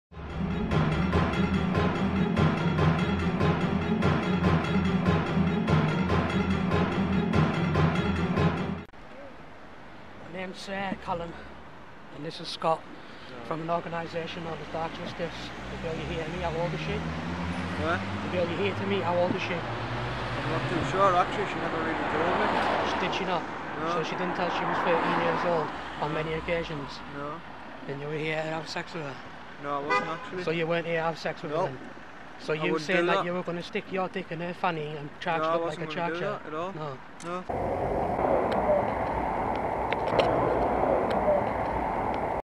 Dark Justice : Bradley Masterson caught trying to meet 13 year old girl
THIS VIDEO DOES NOT BREACH ANY OF YOUTUBE'S RULES AND YOU CAN SEE THE OFFICIAL PRESS RELEASE FOR THIS CASE